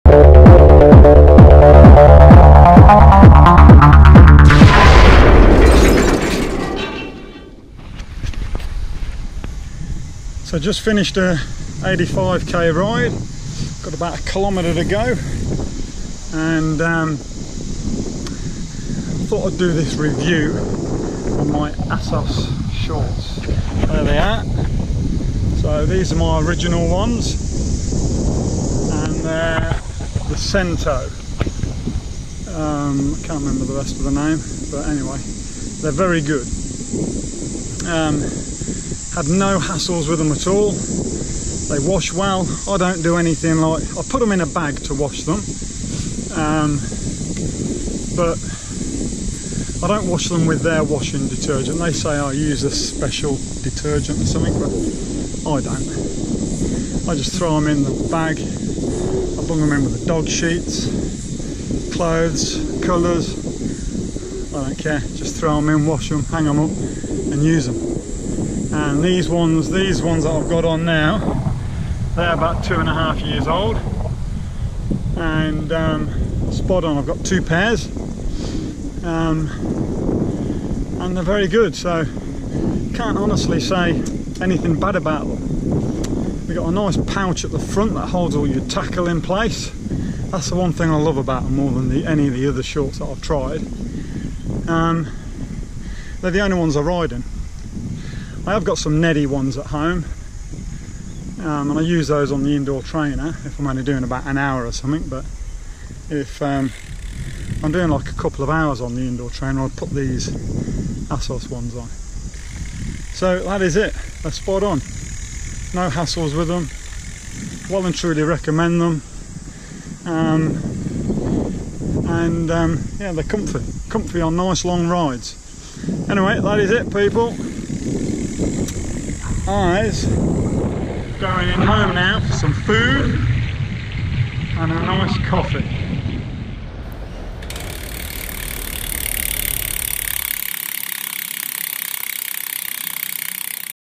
Riding Update Review Of My Assos T-Cento S7 Bib Cycling Shorts
This is an update review of my assos cycling shorts i've been riding with these now for 2 and a half years.
They are still in great shape no tears or rips and the elastic is still in good shape.
They are a very comfortable pair of bike short for short ans long distance ride.
Not the cheapest cycling short out there but they are definitely quality in terms of material the way they are made.